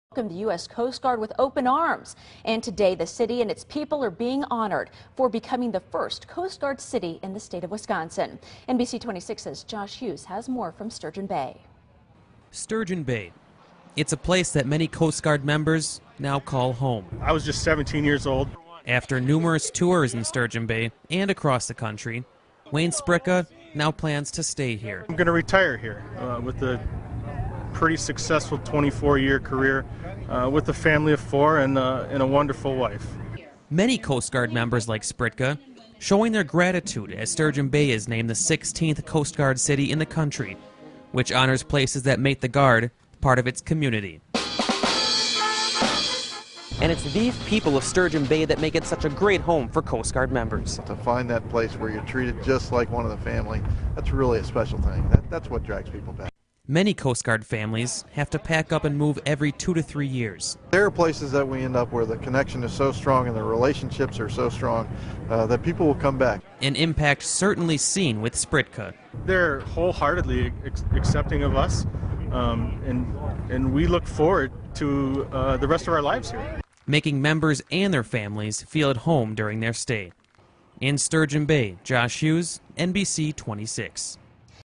Sturgeon Bay Named "Coast Guard City"
It's a place that many Coast Guard members now call home.